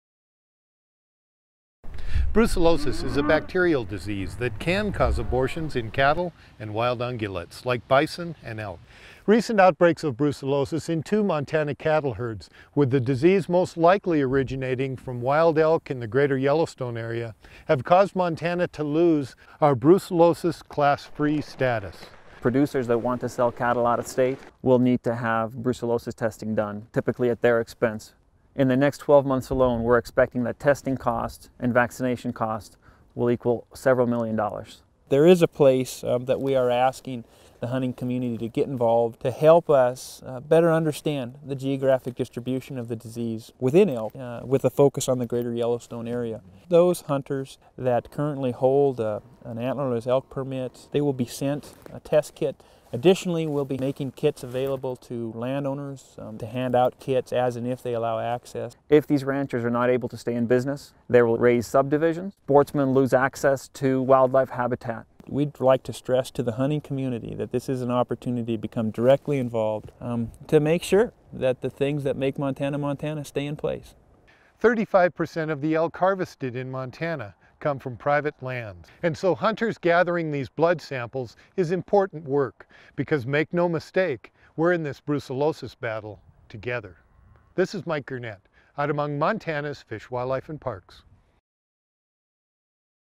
Brucellosis Testing
Hunters, outfitters & landowners help Montana  understand brucellosis.